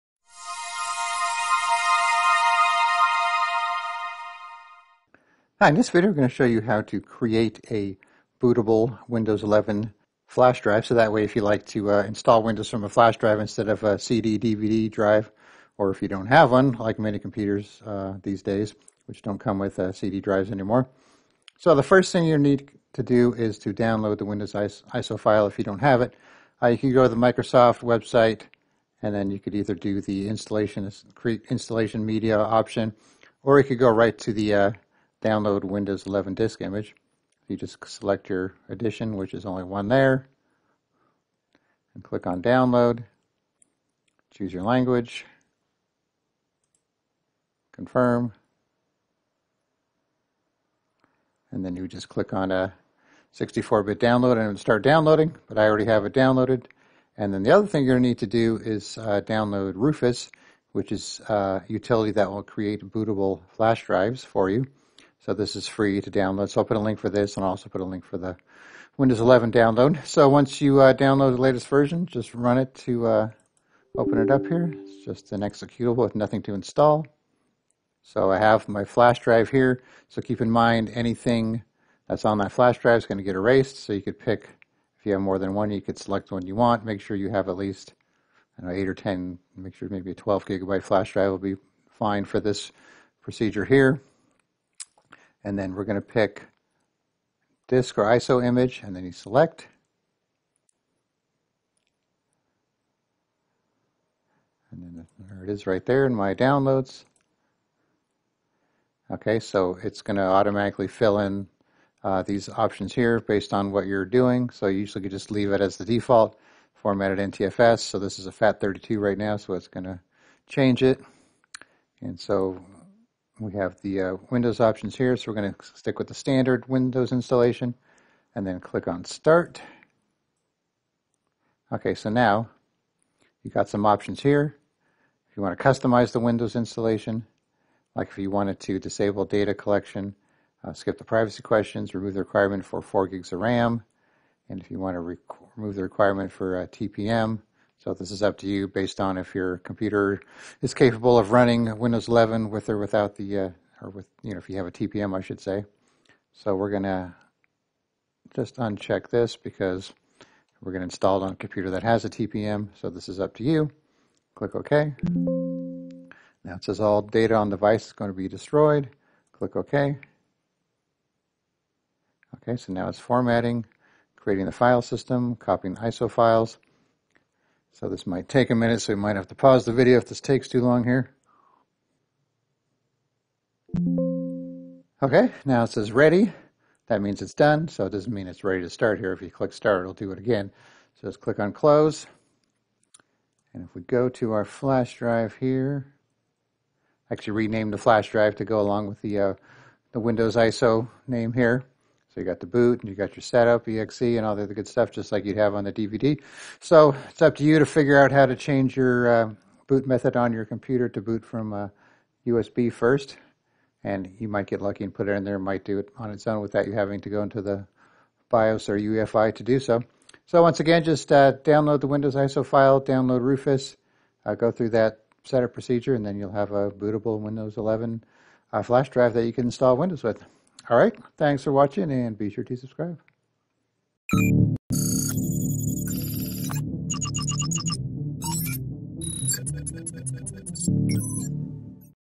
How to Create a Bootable Windows 11 USB Flash Drive to Install Windows & Bypass the TPM Requirement
If your computer doesn't have a CD\DVD drive, then you will need to use another method to install Windows such as booting from a bootable USB flash drive. Fortunately, it's easy to download the Windows 11 ISO installation file and create a bootable USB flash drive using a free tool called Rufus. You will also have the option to remove the requirement for secure boot and TPM, as well as the requirement for needing 4GB of RAM and 64GB of storage space.

Here is a link to download Rufus.

Here is a link to download the Windows 11 installation media ISO file.

Check out our book titled Windows 11 Made Easy and take your Windows skills to the next level!